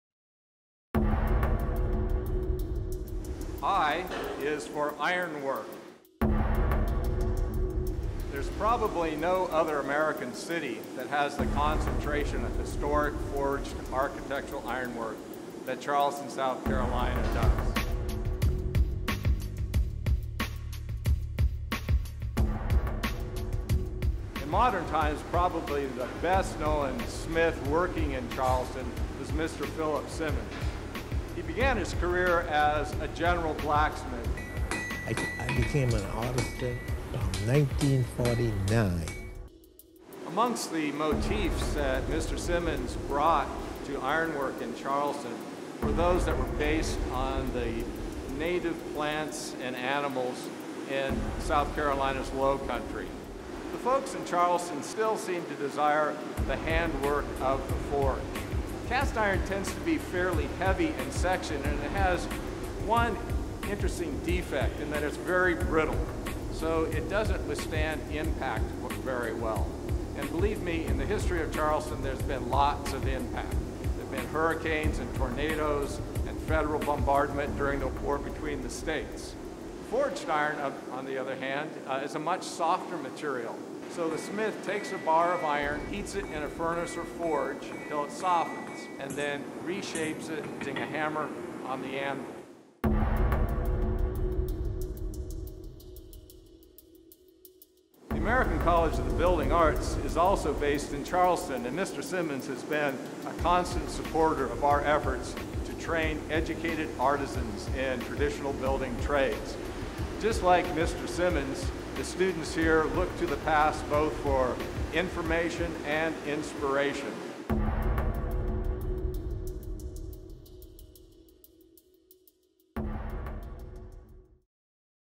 I is for Ironwork
Charleston, SC blacksmith Jay Close talks about ironworking.

DID YOU LIKE THIS STORY?